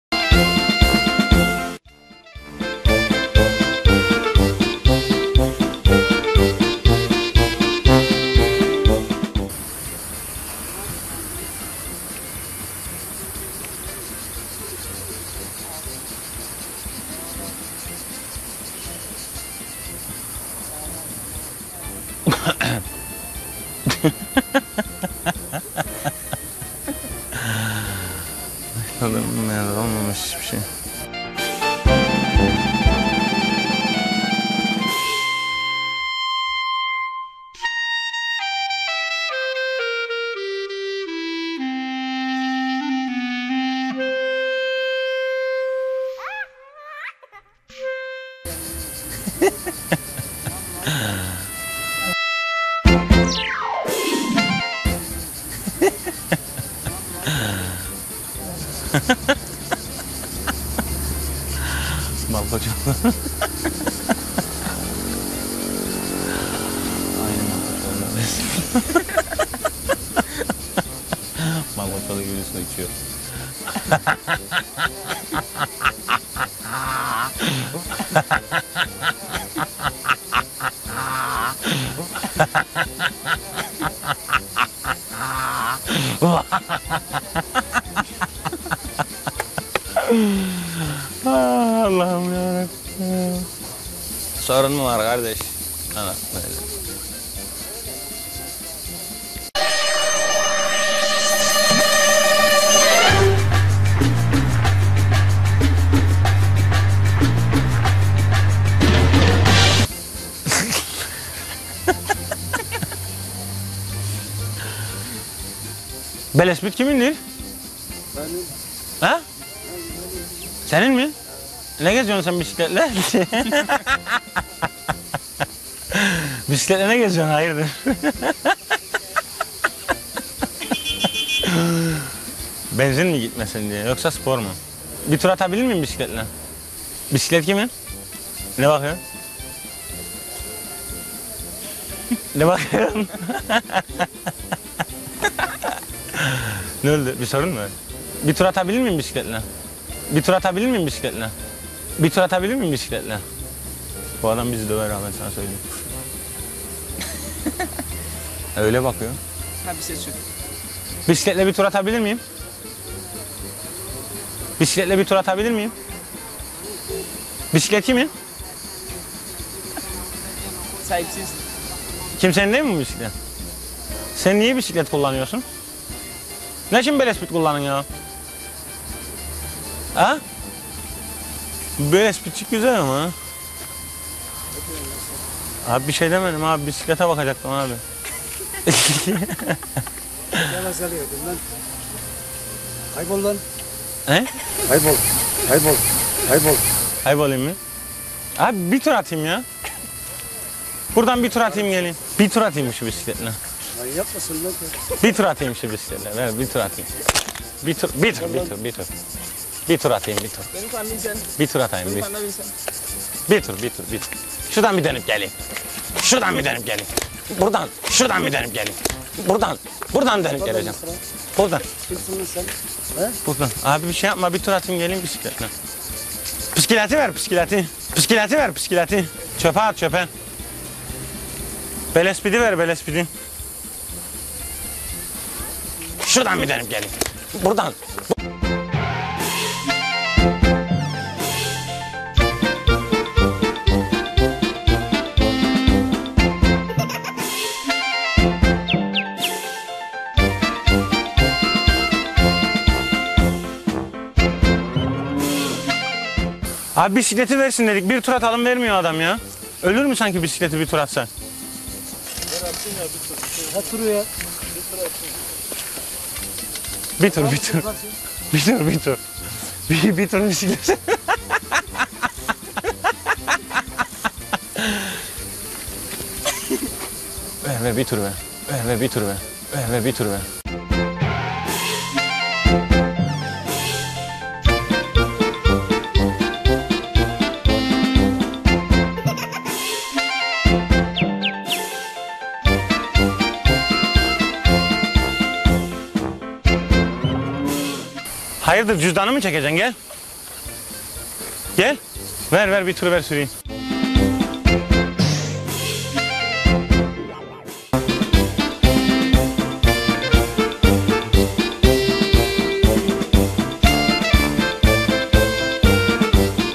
Suratına bakıp gülünce
Şakacı Çağlar sakince bankta oturan şakazedenin suratına bakıp bakıp gülünce bakın neler oluyor...